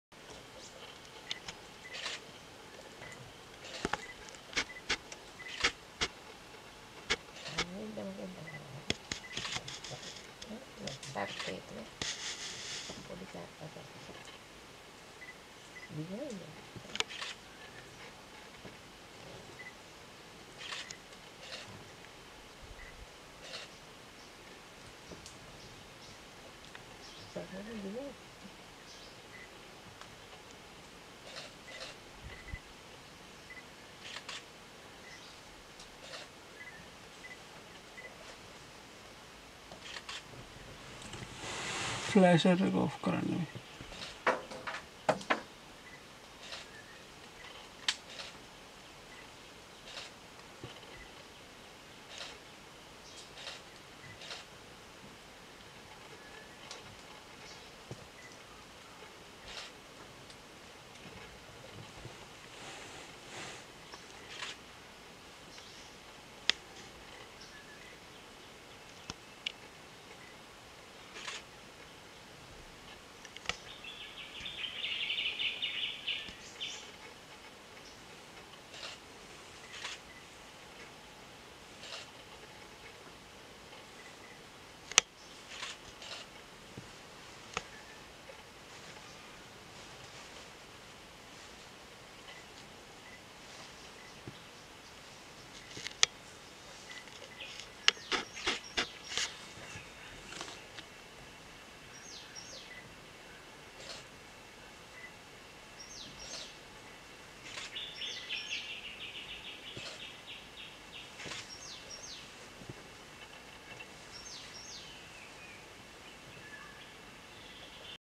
Eating green vine snake
Crested serpent eagle eating a green vine snake Wilpattu national parak srilanka crested serpent eagle is snake killer serpent eagle like to eat snaks